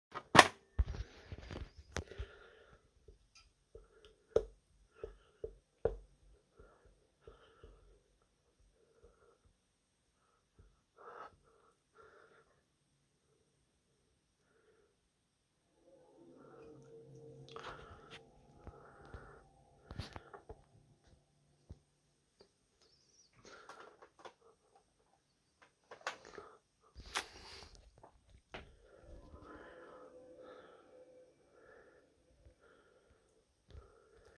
PS2 RSOD (After Main Menu)
I Put a PC Disc in just to get this